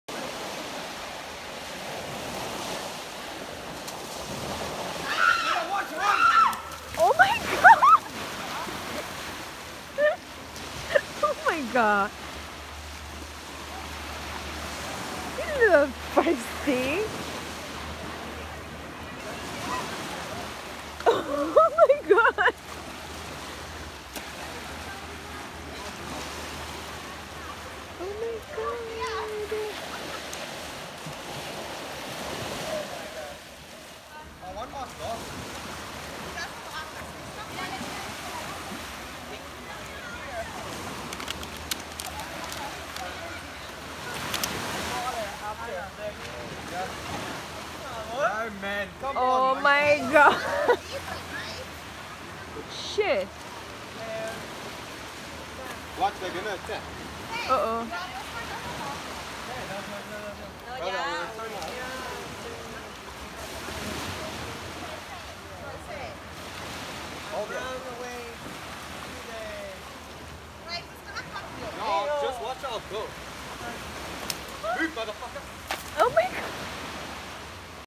Getting Robbed in Monkey Beach, Ko Phi Phi Island | Monkey Attack l Thailand Travel Video
Don’t bring your stuff on monkey beach and if you do , make sure not leaving them unattended because monkeys will steal your stuff lol
In case monkeys attack : you run into water or throw them sand so they leave you alone